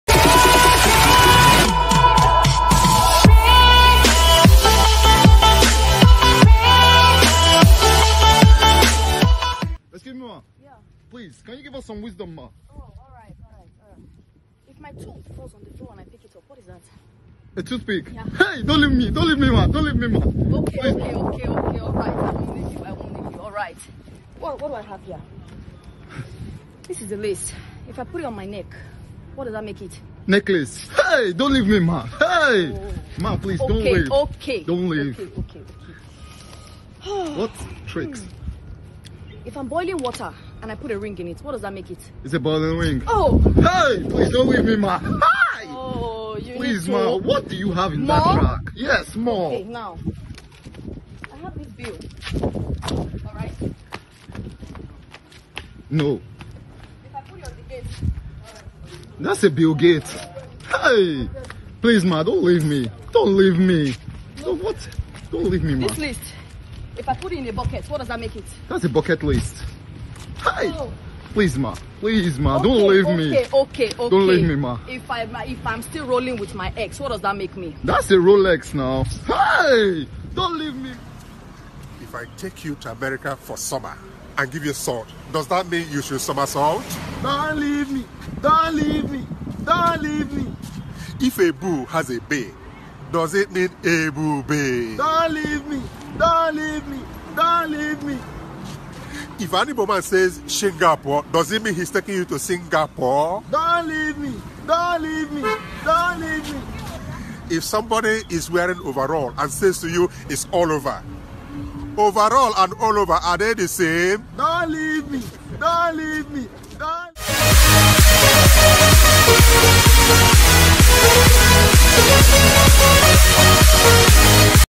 Don't leave me nollywood stars
Nollywood stars doing the don't leave me challenge.